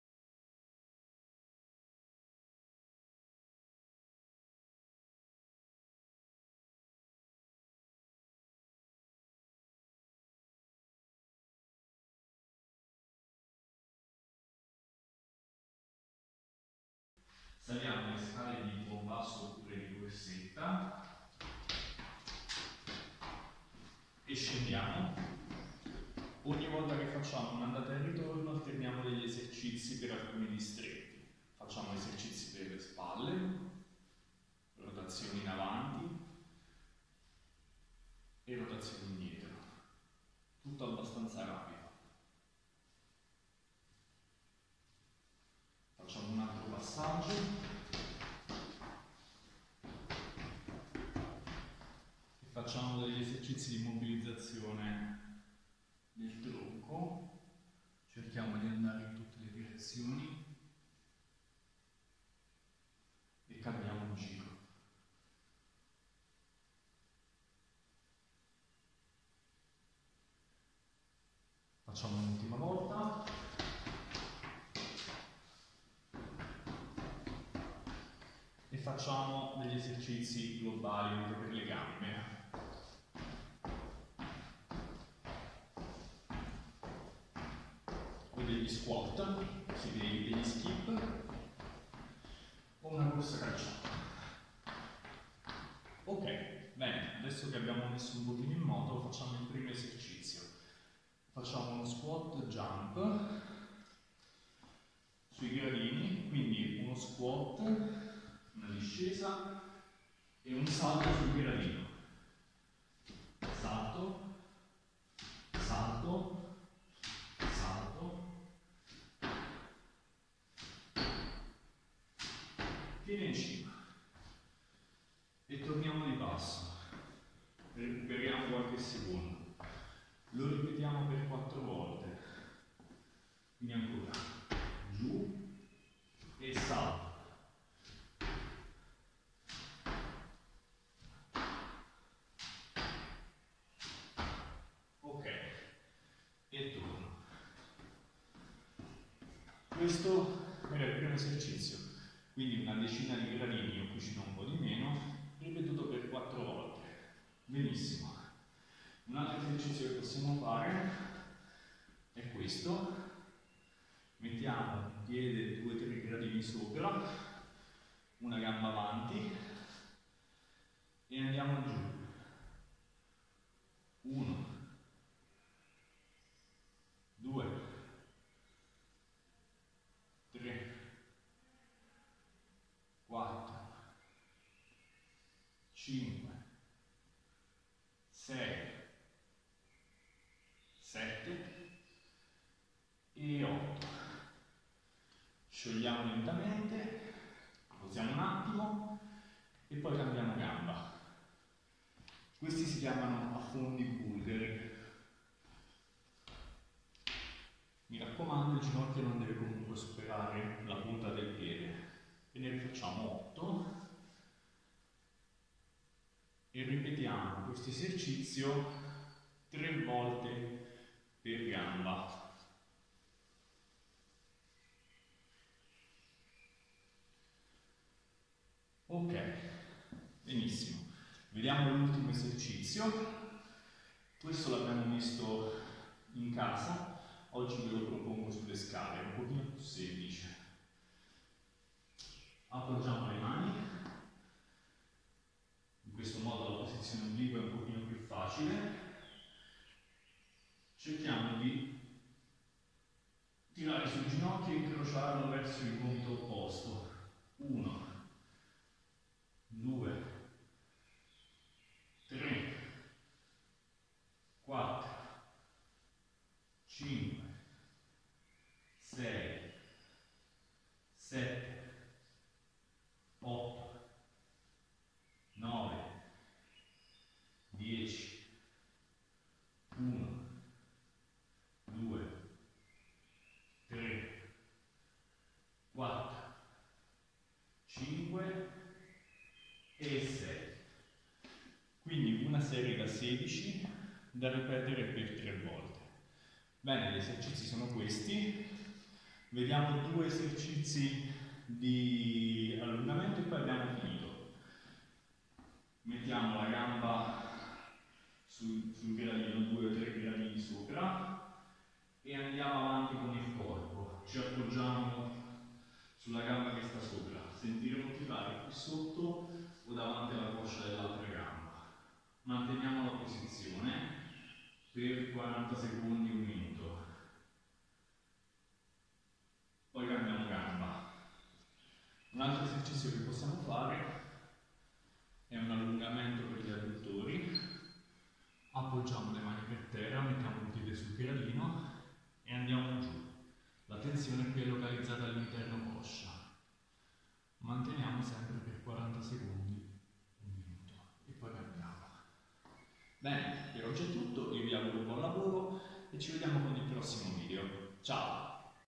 ASD U.S. Acli Nicola Tritella Move Week
In occasione di Move Week, Settimana europea del movimento, lASD US Acli Nicola Tritella propone un video di esercizi sulle scale a cura del prof. Alessandro Esposto.